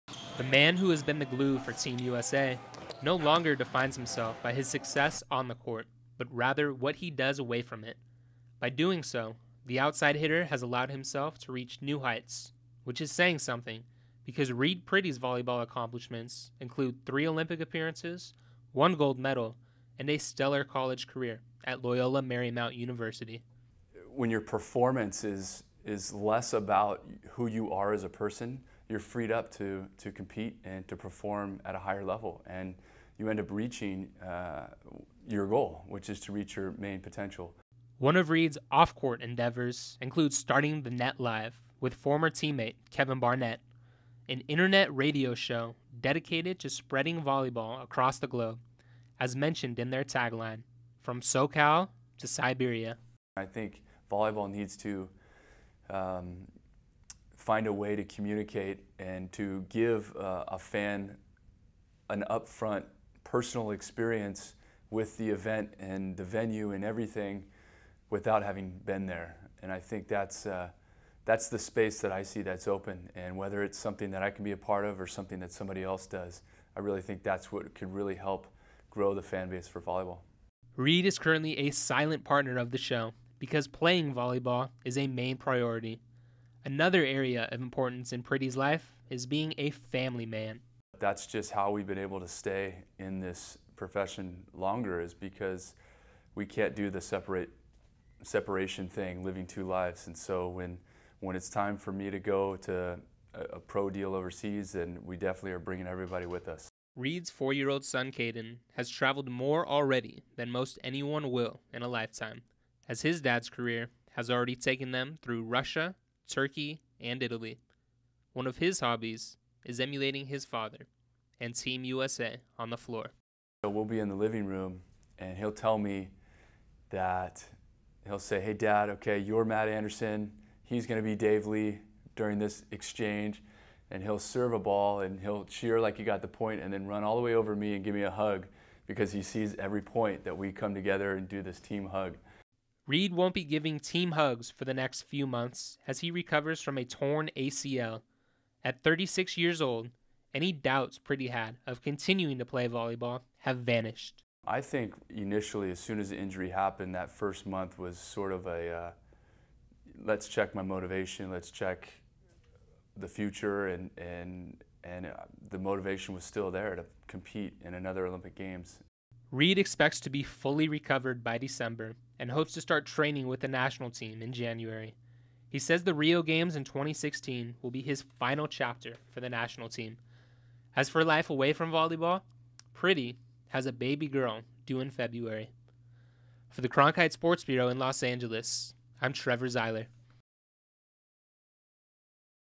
Meet Reid Priddy, outside hitter for USA Volleyball
Meet Reid Priddy, outside hitter for the USA Volleyball Men's National Team. Video by ASU Cronkite School. For more USA Volleyball videos visit our